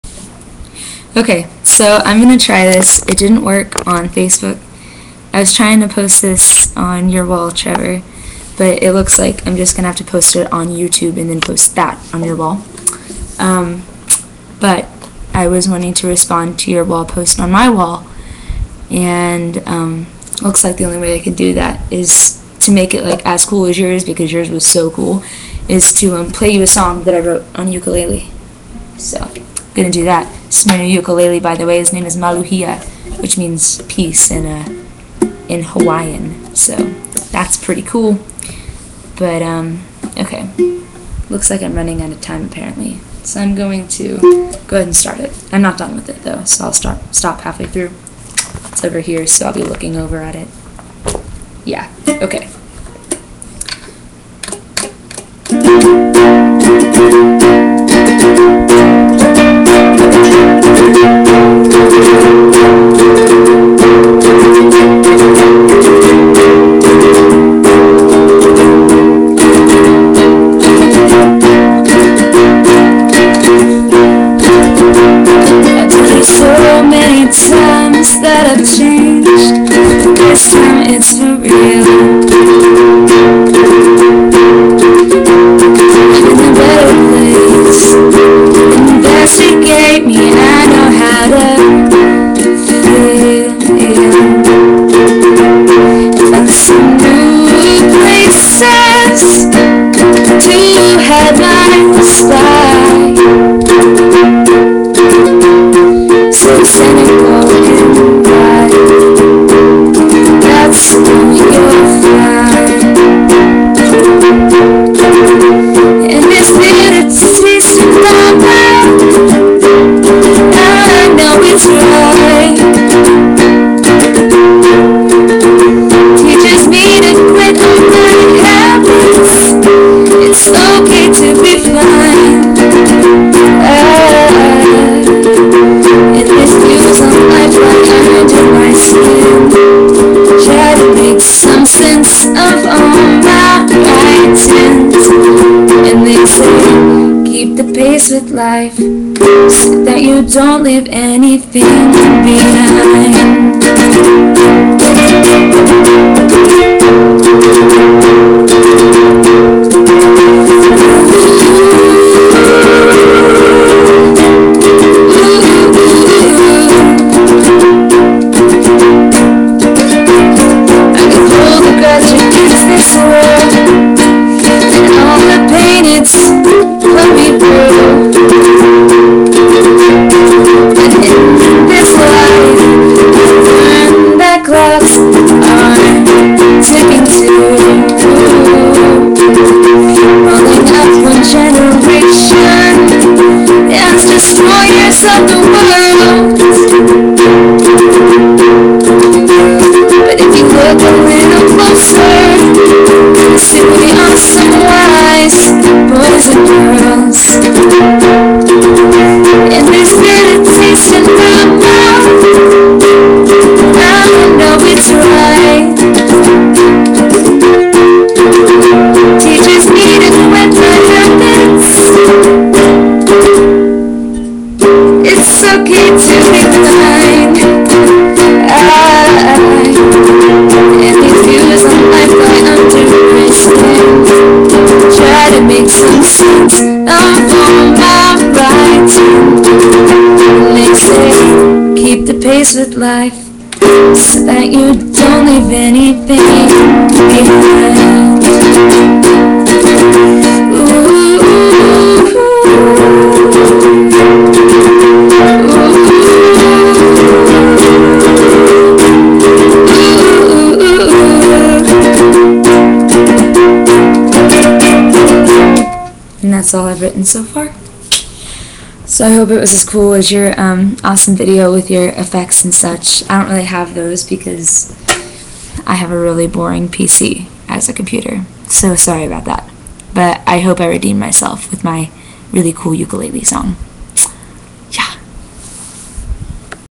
For Trevor Slack:)
My Ukulele Song! Sorry for the horrible horrible quality :(

Lyrics:

I've told you so many times that I've changed,
But this time it's for real
I'm in a better place
Investigate me, I know how to feel

Found some new places
To hide my spite
So cynical in life
That's all you'll find

And this bitter taste in my mouth
I know it's right
Teaches me to quit all my habits
It's okay to be fine
And my views on life lie under my skin,
Trying to make some sense of all my writings
And they say," Keep the pace with life, so that you don't leave anything behind.
Ooh Ooh

I could hold a grudge against this world
And all the pain it's put me through
But in this life, I've learned that clocks are
Ticking, too

Balling up one generation
As "destroyers of the world"
But if you'll look a little closer,
You'll see we are some wise boys and girls

Ooh Ooh